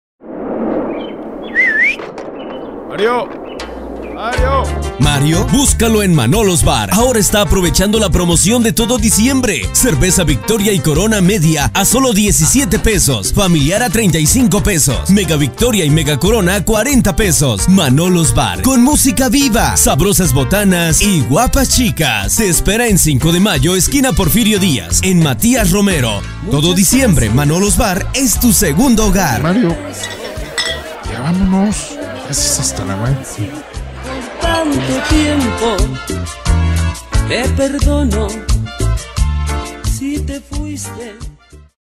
MANOLOS BAR promocion decembrina
SEGUNDO SPOT PARA UNO DE NUESTROS CLIENTES CON MAS FRECUENCIA EN NUESTRA AGENCIA,,MANOLOS BAR,,Y AHORA CON SU PROMOCION DE FIN DE AÑO